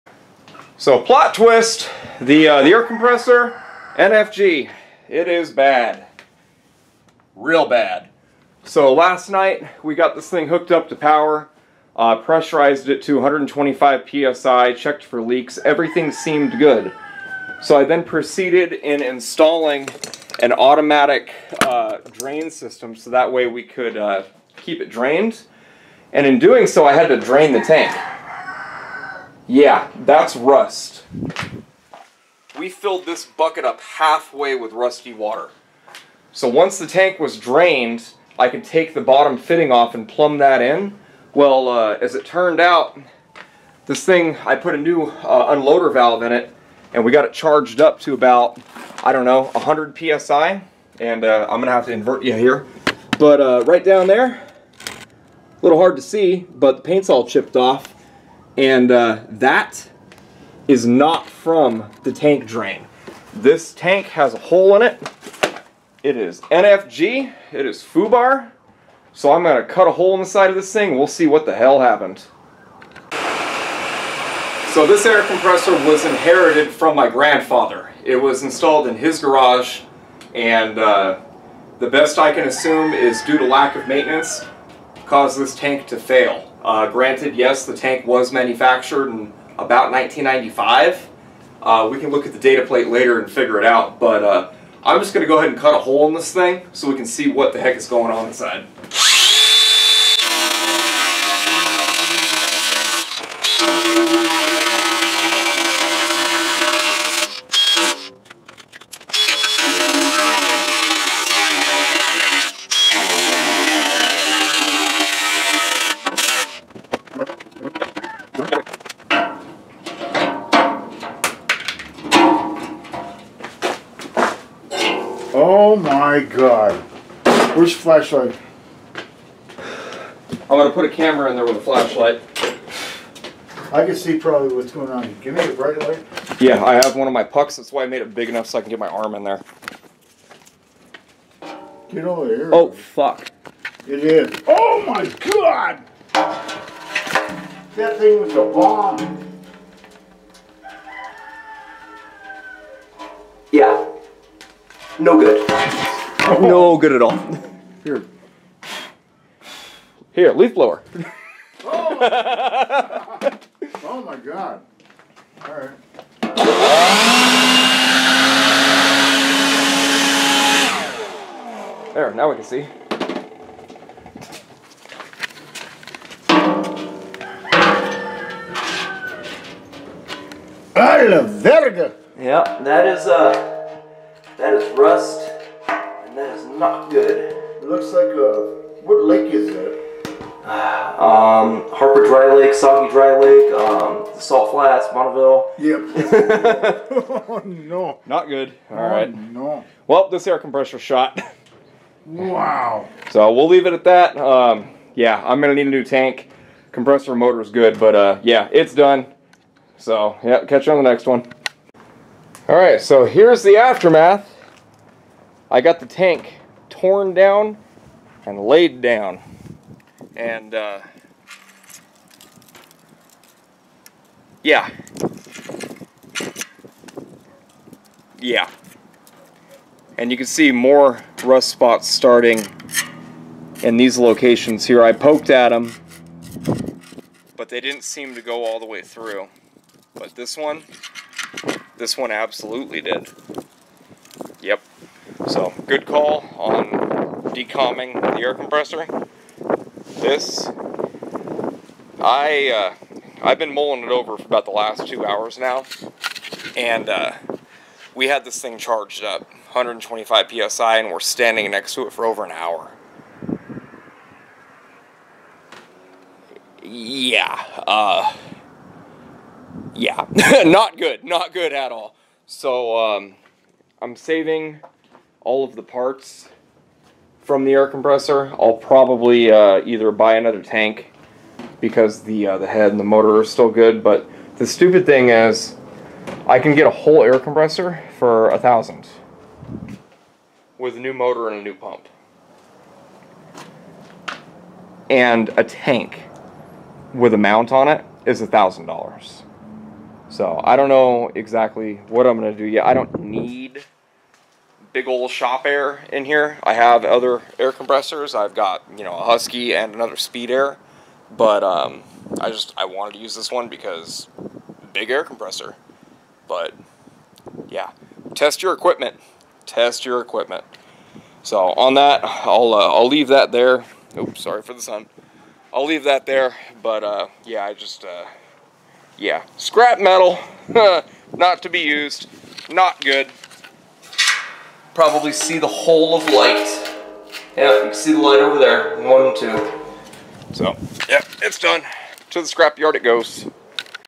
Failed Air Compressor! Hole in tank, disaster averted.
Long story short, I hooked up my grandfathers air compressor from his shop, and... NOT GOOD. Tank was leaking water from what I thought was the drain, so I bought an automatic drain setup and the leak found a new way out. THROUGH THE TANK ITSELF. Tank rusted through, lack of maintenance caused this failure. Glad that it failed the way that it did. It was ready to rupture, I found cracks on the outside and inside of the tank showing it was ready to rupture.

Ever heard someone talk about an air compressor that ruptured in their presence? Neither have I. Close call, for sure. Absolute insanity - Seconds away from  certain death.

Just make sure you drain your air compressor. Pressure vessels aren't to be taken lightly.

When the tank failed I was about 50 feet away and heard the air start releasing. I thought it was the drain hose that ruptured, so I ran into the garage and hit the breaker for the pump to stop building air and reassess. Once the pump stopped I went to the tank (approx 100psi at this point) and shut the drain valve. didn't change the air leak, and as I was removing my hand I felt the stream of air coming from the tank.

I ran out of the garage and assessed the situation - having a 100psi BOMB in my garage, and risking the destruction of my new garage and the back windows of my house, I ran back in with a zip tied blow gun and chucked it up to the compressor to help bleed the tank down.

Once the tank was below 20psi I reapprocached and assessed the situation, with a camera in hand. Rest is on the video.